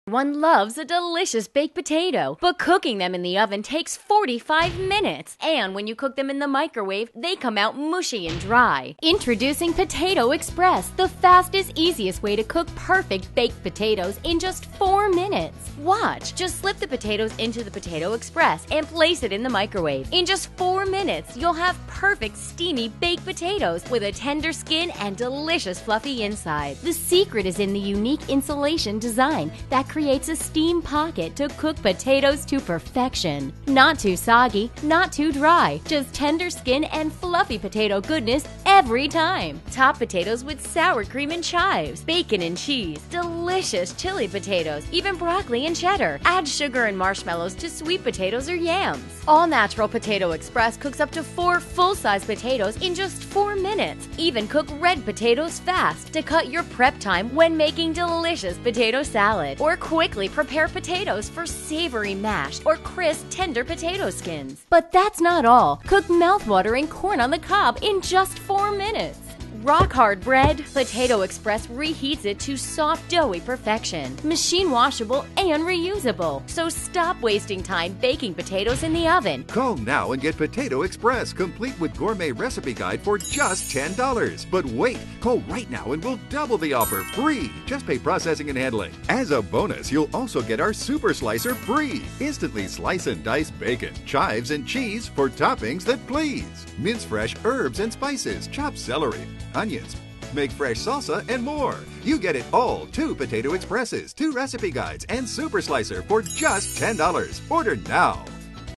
Potato Express - Perfect Baked Potatoes in Only 4 mins!
Perfect Oven Baked Potatoes in Just 4 Mins!